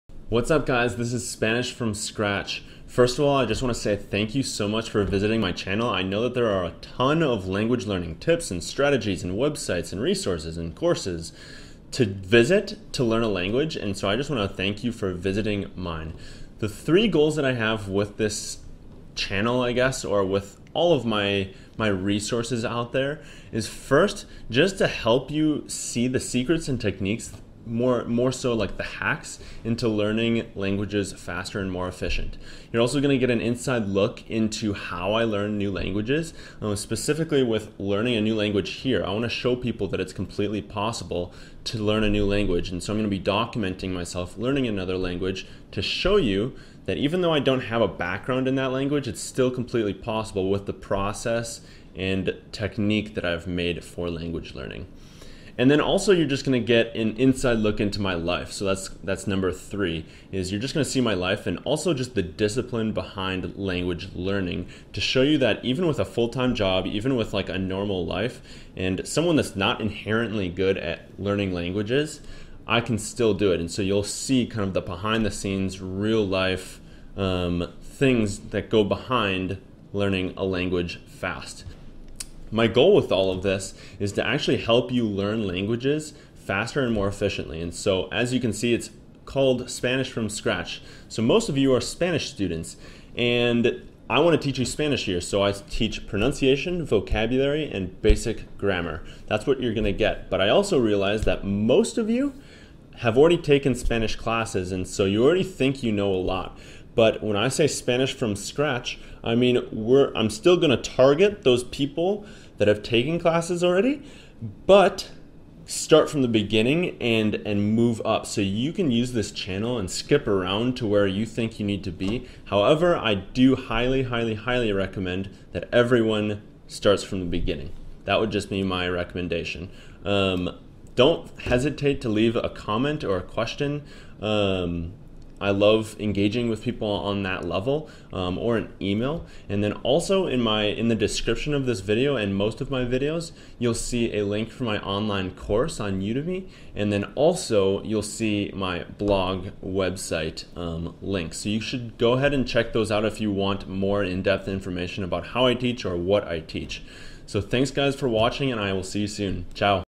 Spanish from Scratch - Introduction
Welcome to Spanish from Scratch - where you will find consistent language learning videos that bring you behind the scenes in my language learning process!.

I also have an online course for you to take.